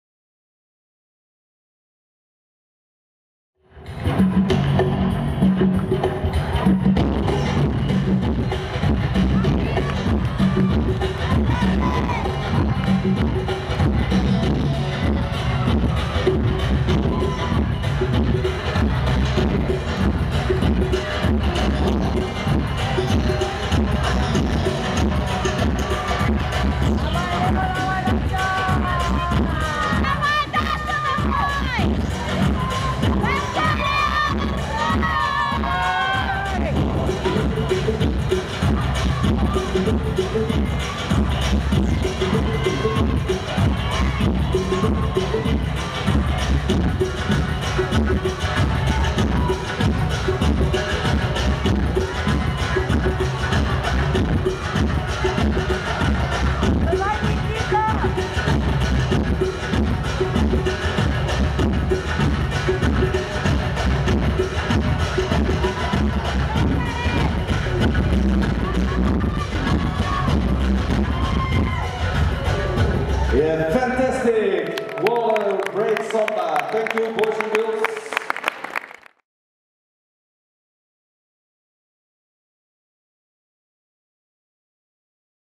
Joshua Bonici - Elisaveta Chelnakova, RUS | Disney 2018 - World U12 LAT - SF S
Disney 2018, Disneyland Paris/France - WDC AL World Championship U12 Latin

Share and enjoy! Subscrib to channel for more! Thank you for your support!

RESULTS:
1. Alexandr Kovalev - Alisa Volper (224)  Russia
2. Daniel Novikov (239)  U.S.A.
3. Joshua Bonici - Elisaveta Chelnakova (259)  Russia
4. Nikita Parkhomenko - Oleksandra Iarmoliuk (277)  Ukraine
5. Leo Kolmykov - Daria Andreyev (264)  U.S.A.
6. Antonio Esposito - Mariarosaria Dal Monte (228)  Italy